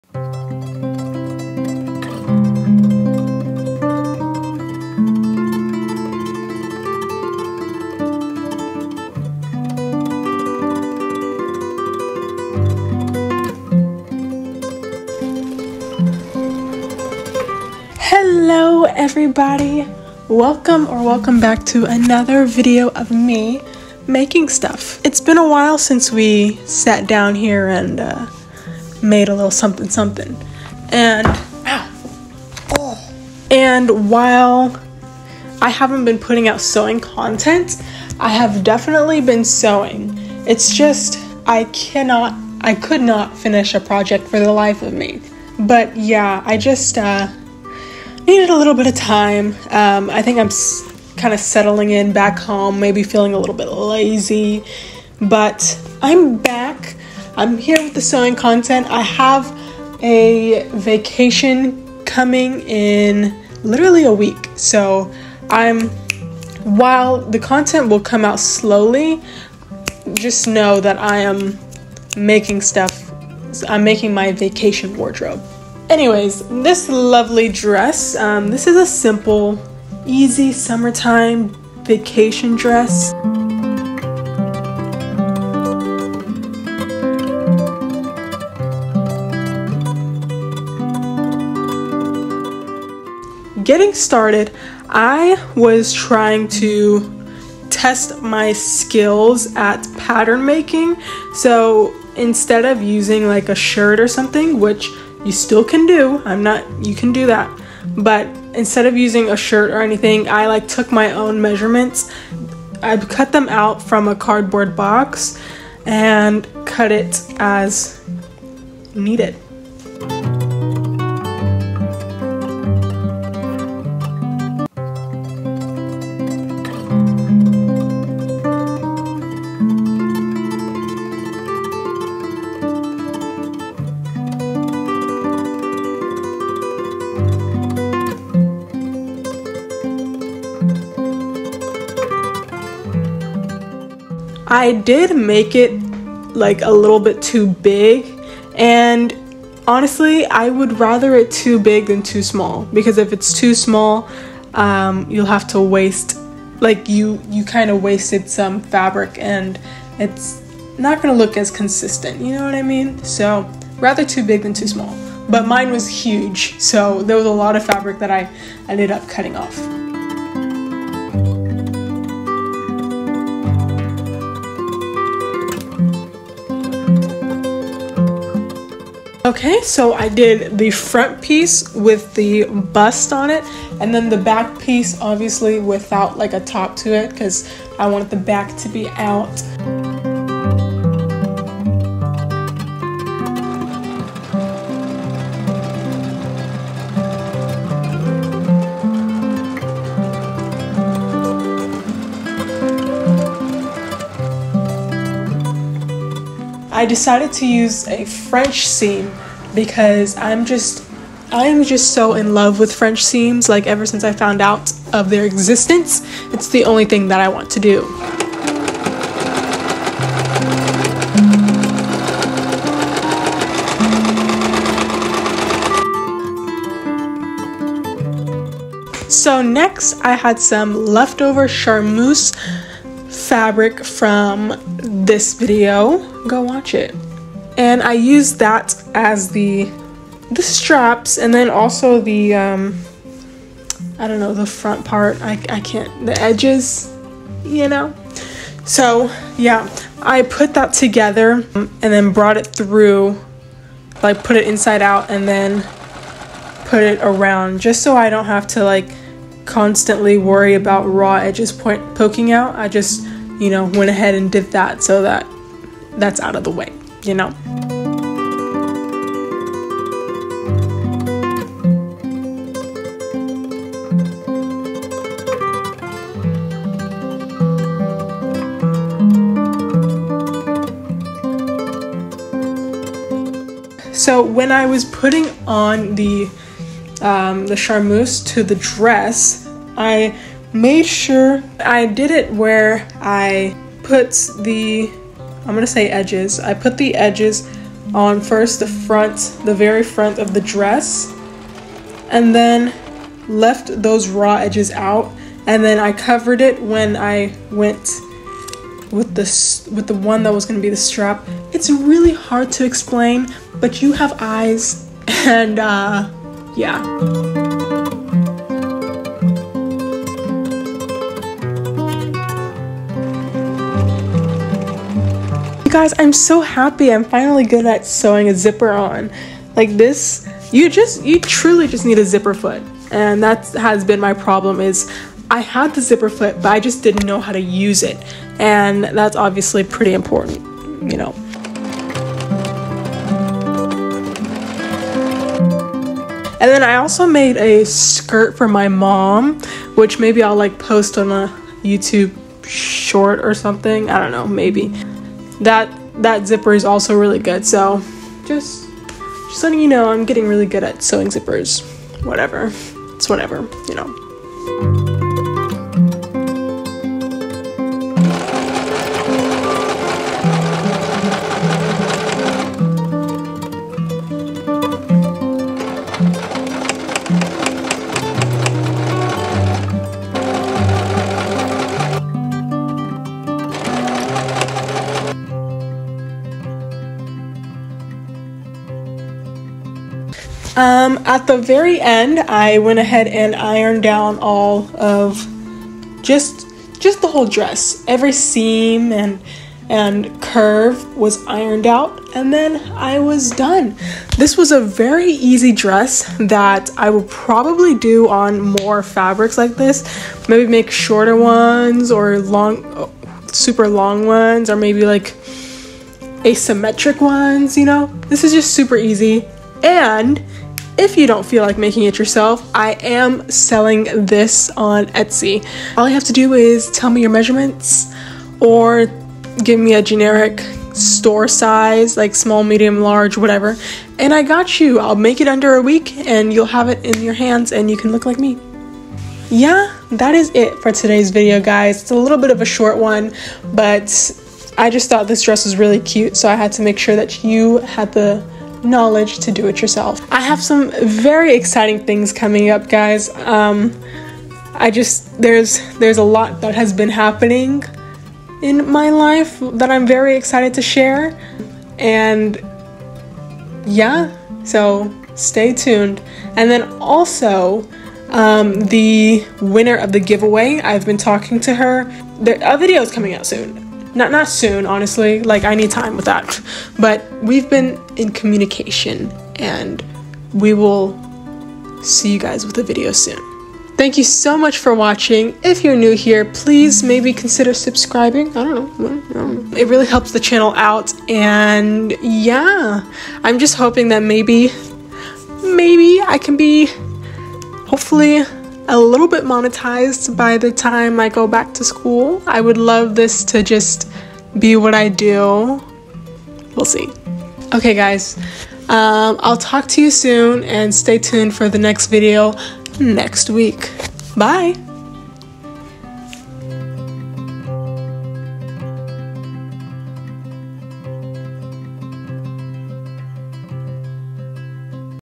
DIY Vacation Dress| Easy Sewing Tutorial| Maxi Dress Tutorial| How to Vacation Wardrobe
Hey guys!
This is a DIY Maxi Dress tutorial for my vacation wardrobe! I'm very happy with the way it turned out!
Love,
Alexis